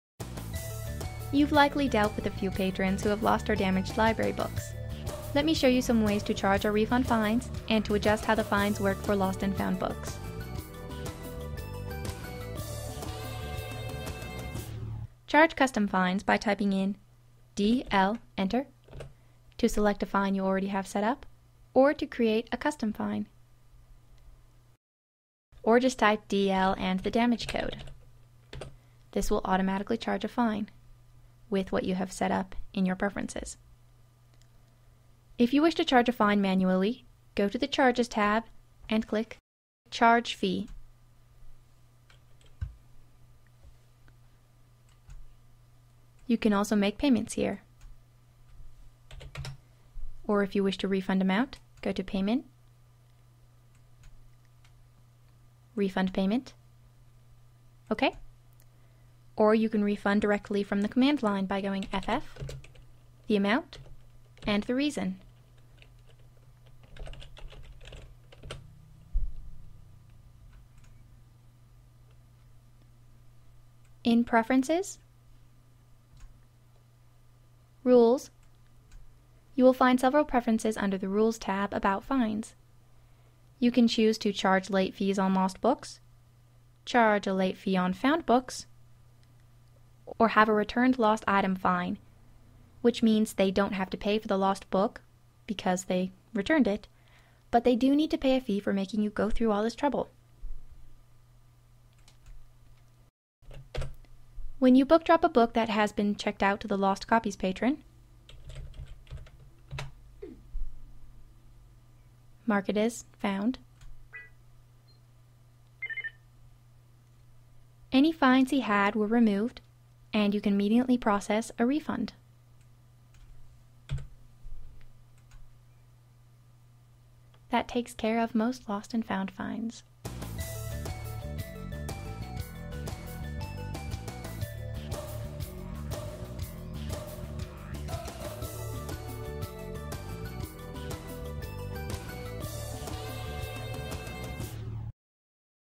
Lost and Found Fines in Alexandria v6
Charging fines, and Rules preferences in Alexandria v6 library management softare.